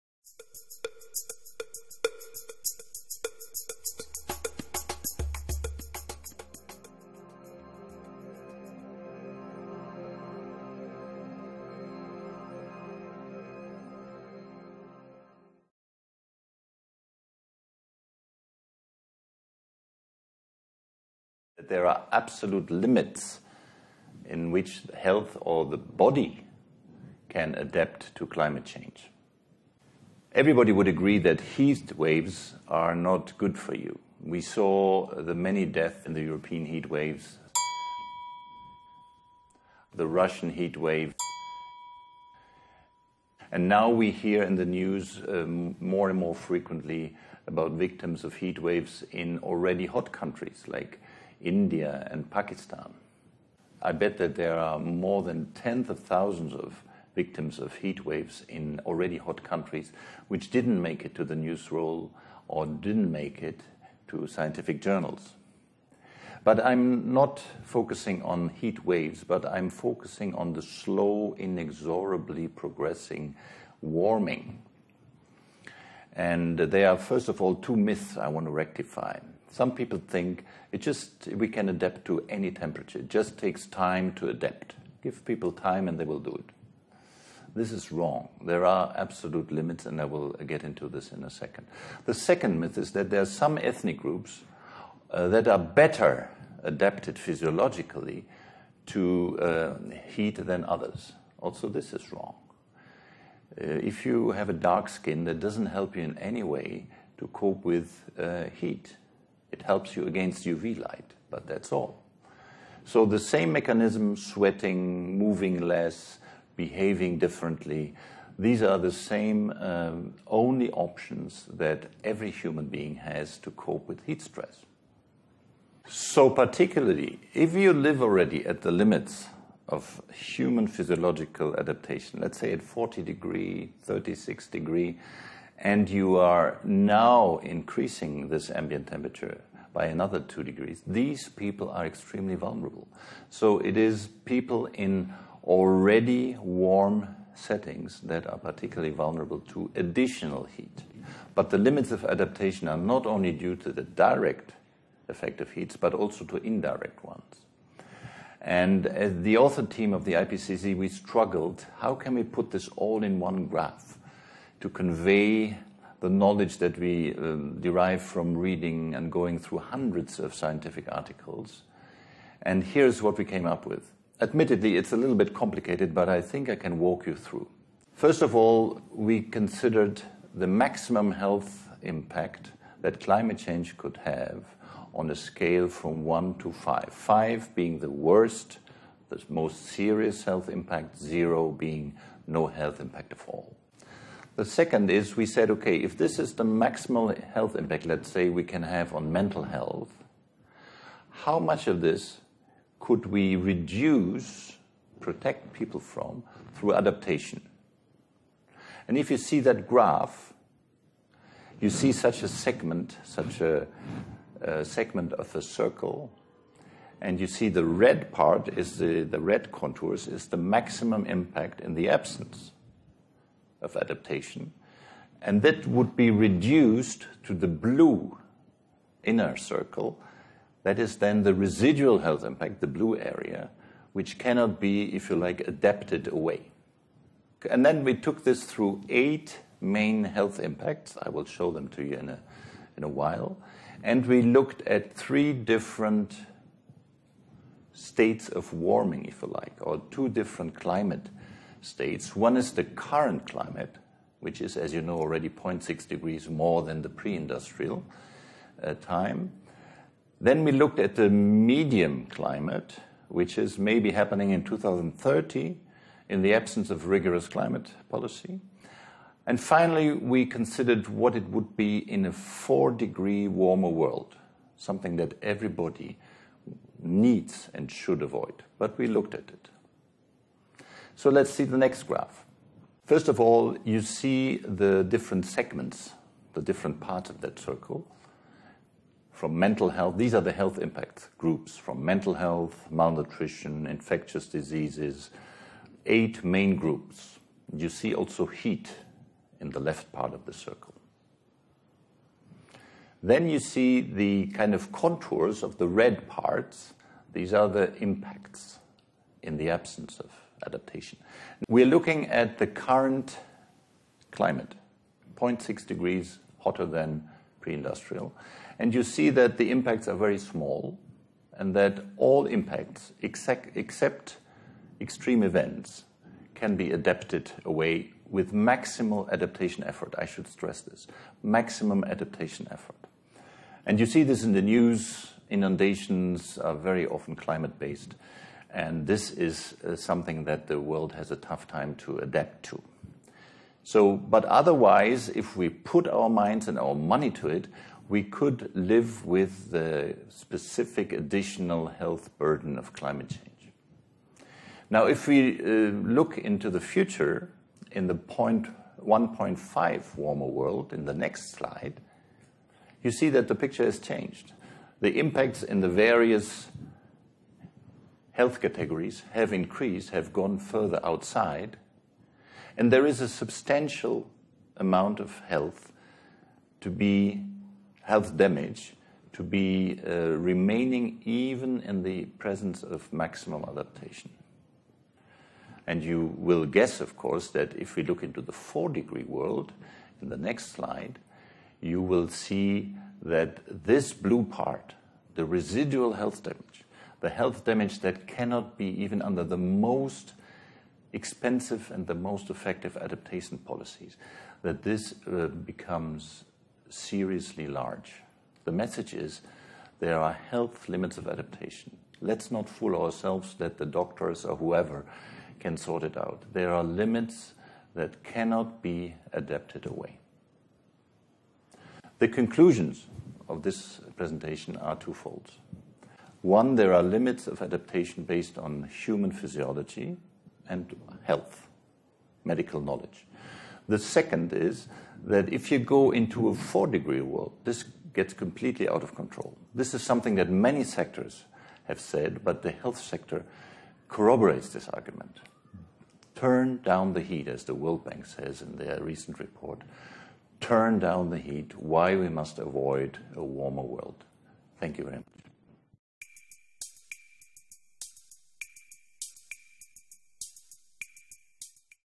The health basis for absolute limits of adaptation efforts keep the 2 c goal
MOOC: Climate Change and Health on iversity.org
Professor Rainer Sauerborn, Heidelberg Institute of Public Health